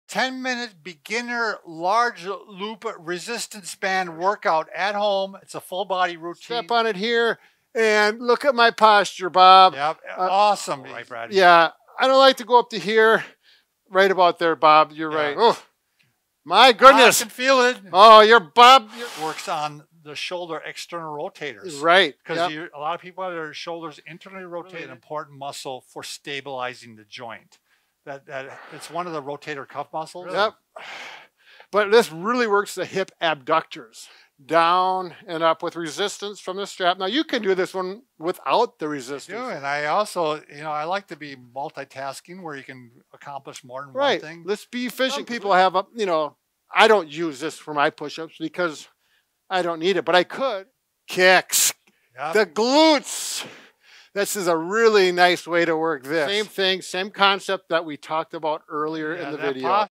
10 Minute Beginner Large Loop Resistance Band Workout (60 Sec Video)
Bob and Brad demonstrate a workout using the Bob and Brad pull up band set.

Welcome to Bob and Brad Clips! The 60 second version of the full Bob and Brad Video. New videos posted everyday!